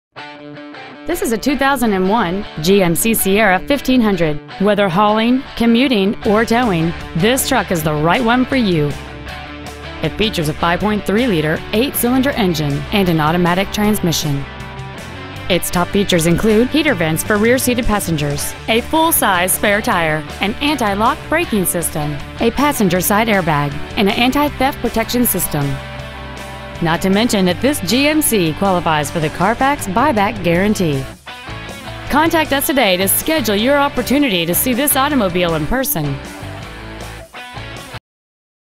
2001 GMC Sierra 1500 OH
Year : 2001
 Make : GMC
 Model : Sierra 1500
 Engine : 5.3L V8
 Trans . : Automatic
 Exterior : Purple
 Miles : 130,593
 Interior : Gray
 Ed Schmidt Pontiac-Volvo-Saab
 26875 North Dixie Highway
  , OH 43551